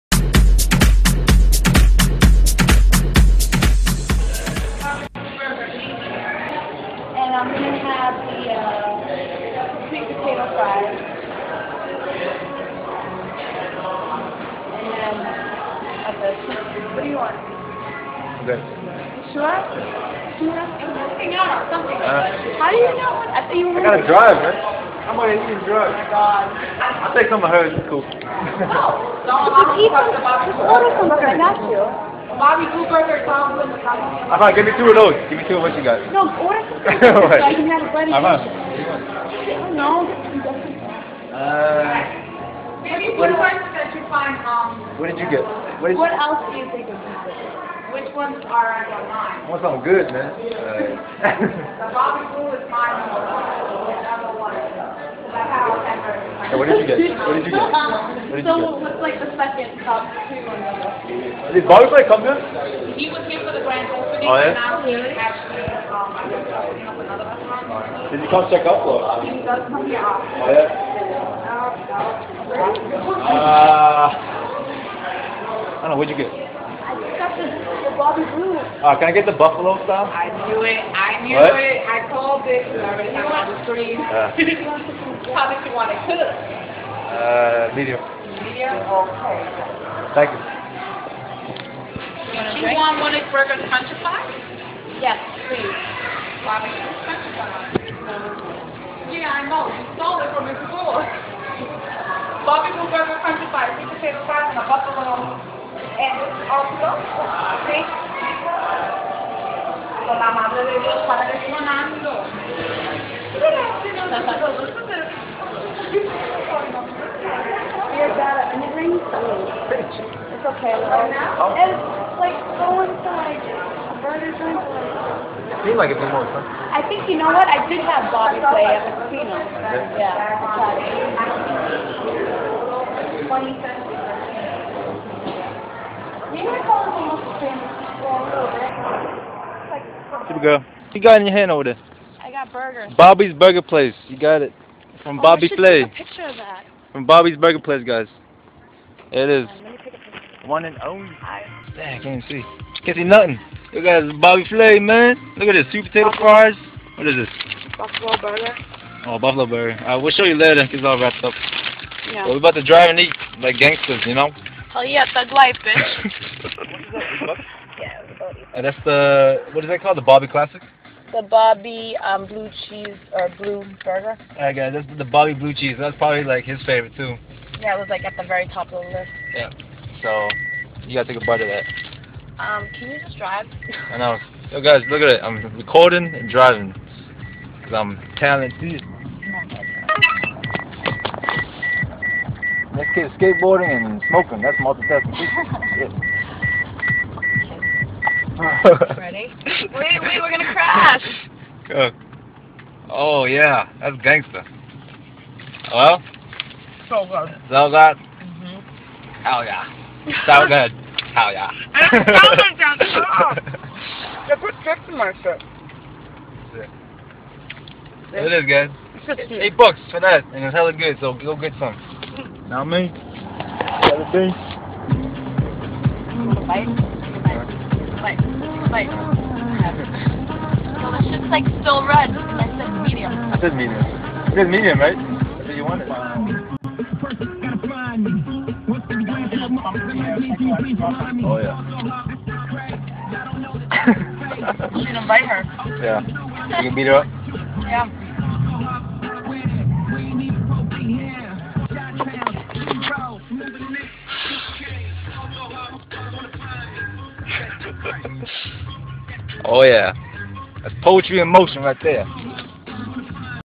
Trying Out Bobby Flay's Burger Palace w/ SuperGirL / Eating Out
We ordered the bobby blue cheese burger and the buffalo burger with sweet potato fries. It was delicious!